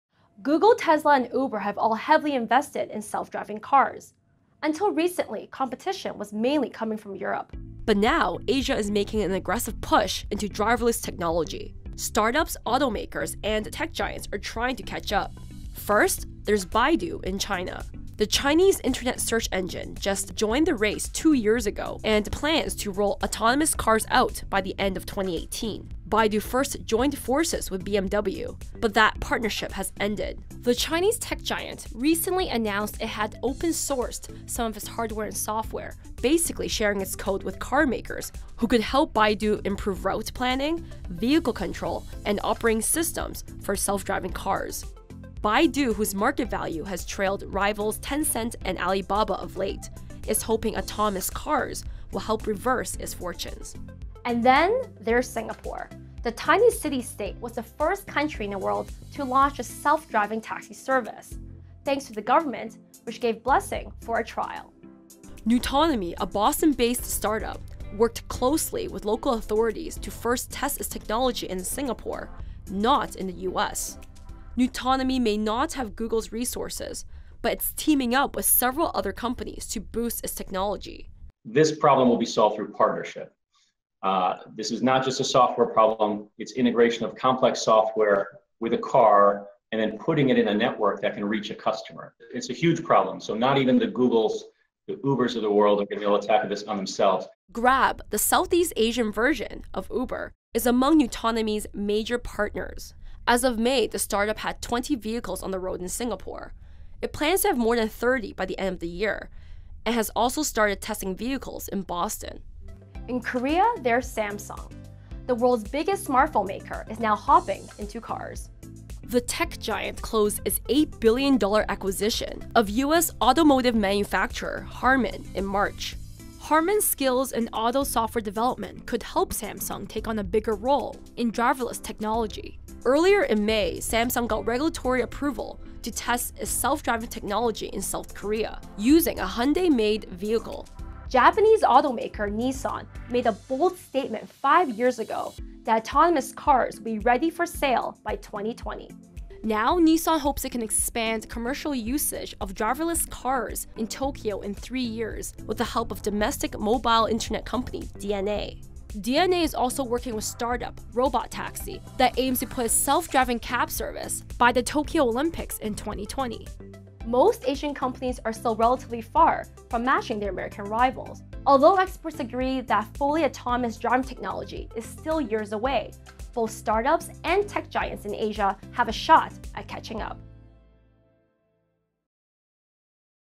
Race for Self-Driving Cars Gears Up in Asia
Asian tech giants, startups and auto makers, including Baidu, nuTonomy and Nissan, are aggressively pursuing driverless-vehicle technology. WSJ's Eva Tam examines how these companies are aiming to catch up to the global industry leaders. Photo: Getty Images